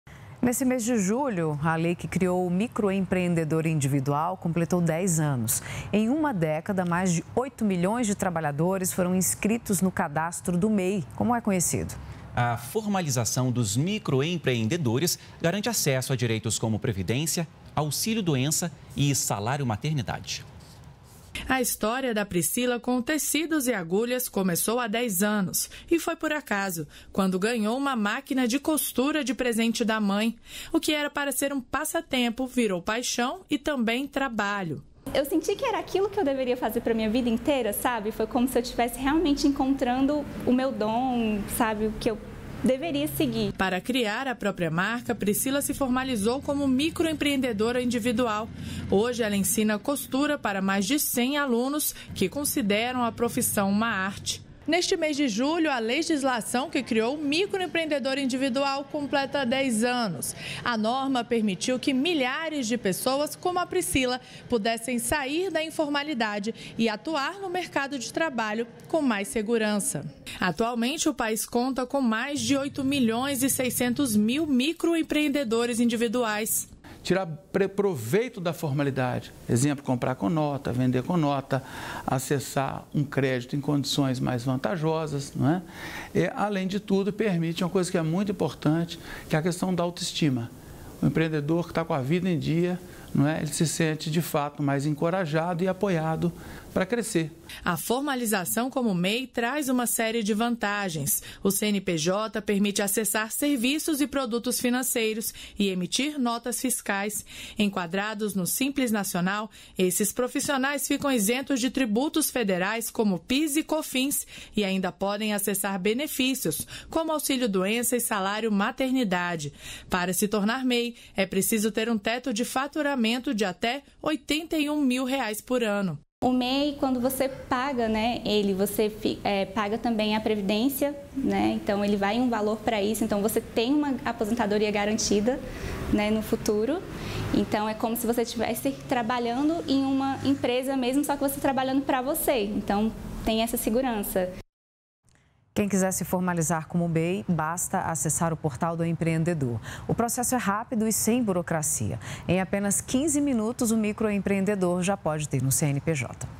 Em Julho, a legislação que criou o MEI completa 10 anos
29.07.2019 -  Em julho, a legislação que criou o microempreendedor Individual, o MEI, completou dez anos. A medida permite a formalização dos microempreendedores e garante acesso a direitos como previdência, auxílio-doença e salário-maternidade.